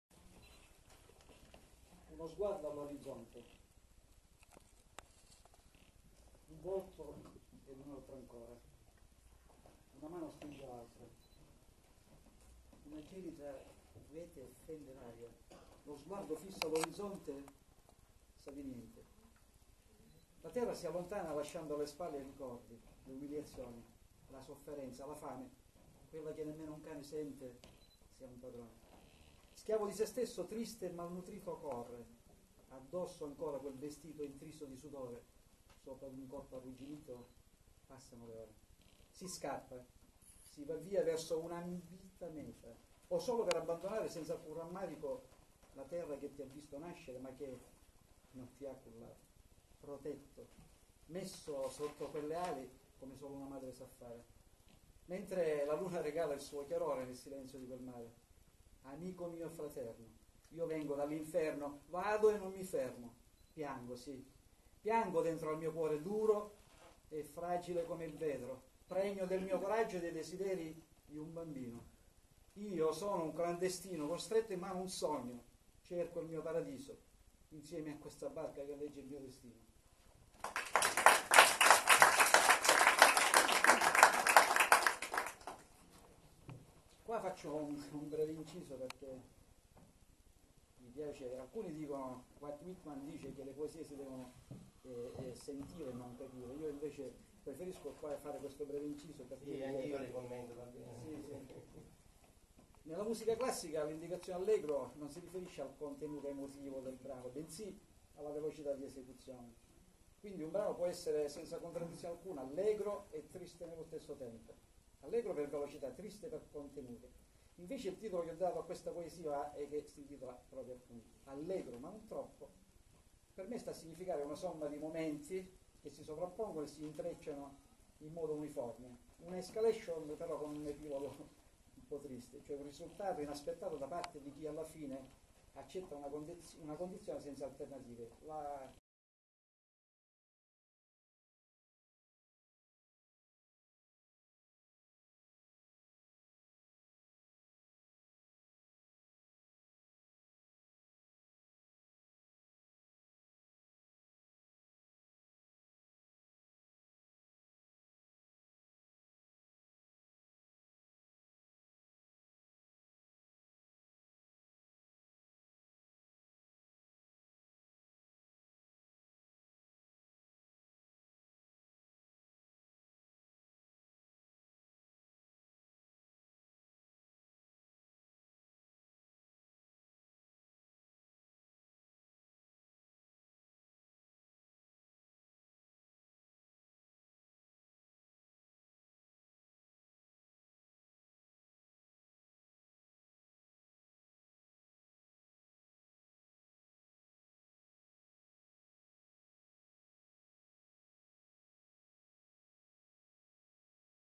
Giornata Mondiale della Poesia - Carmelo Rannisi
21 marzo 2010 - Giornata Mondiale della Poesia

Alla Sala Magma di Catania gli autori leggono i loro componimenti

Serata organizzata da Orazio Indelicato per "Bottega della Poesia" in collaborazione con "Terre Forti - associazione culturale"

In questo video: Carmelo Rannisi

Riprese di Sara Aguiari